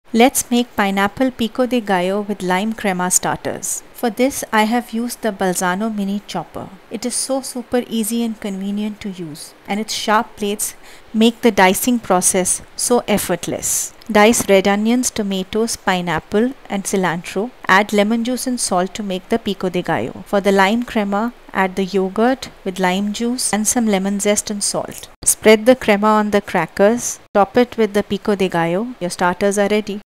Pineapple Pico De Gallo Canapes with Lime Crema | Balzano Chopper
Meet the good taste today with this Pineapple Pico De Gallo Canapes with Lime Crema 😋
Just one touch push and this Balzano Mini Food Chopper makes the hard work of Chopping & Micning just fun and easy 🥰